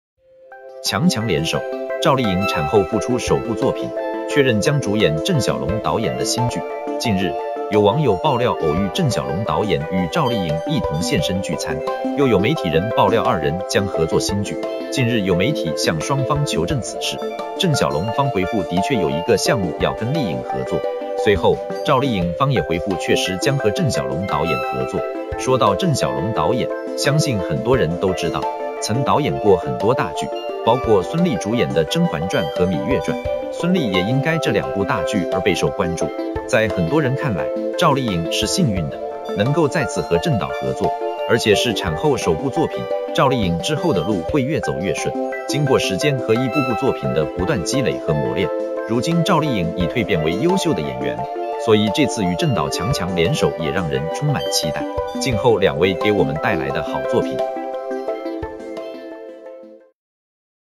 强强联手,赵丽颖产后复出首部作品,确认将主演郑晓龙导演的新剧
➤Thanks收看！